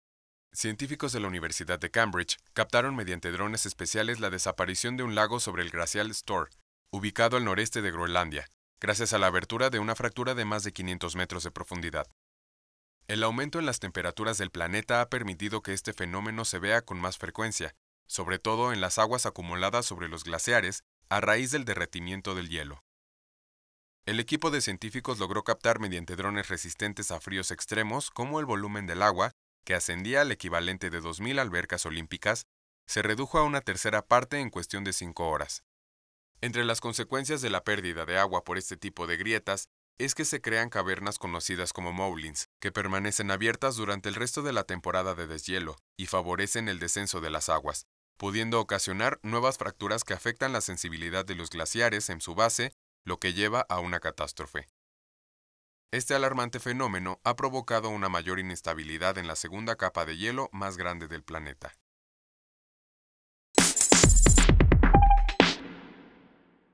Drones captan desaparición de un lago en Groenlandia
Científicos de la Universidad de Cambridge, captaron mediante drones especiales, la desaparición de un lago sobre el glaciar Store, ubicado al noroeste de Groenlandia, gracias a la abertura de una fractura de más de 500 metros de profundidad.